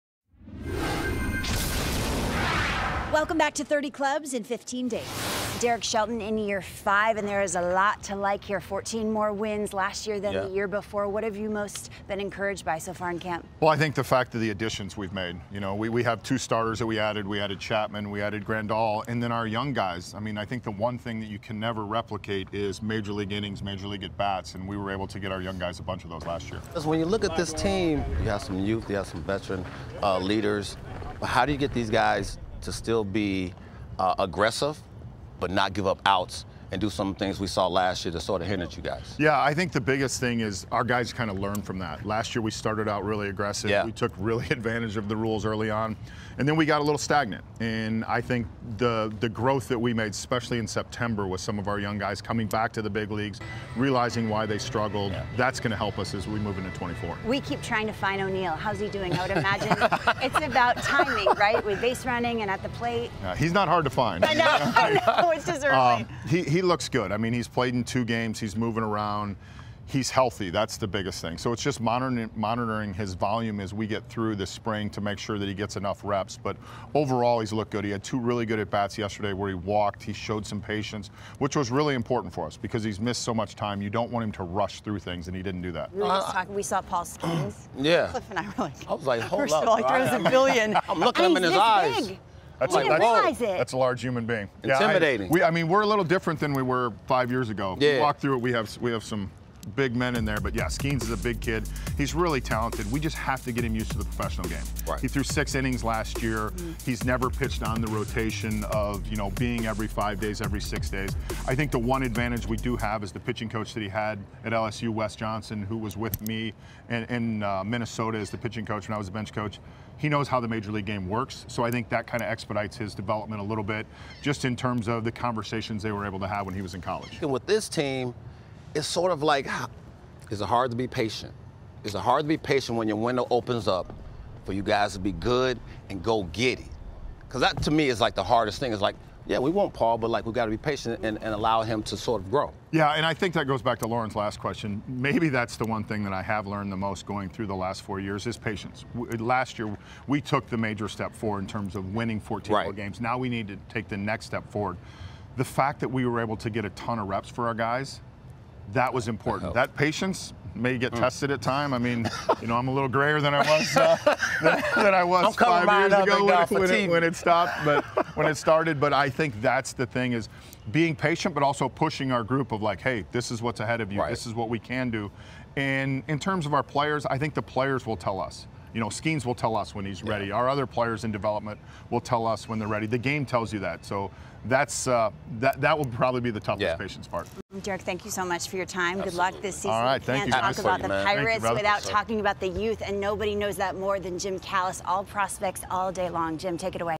30 Clubs in 15 Days: Pirates Manager Derek Shelton
Pittsburgh Pirates manager Derek Shelton discusses the impact of new roster additions, Spring Training so far and much more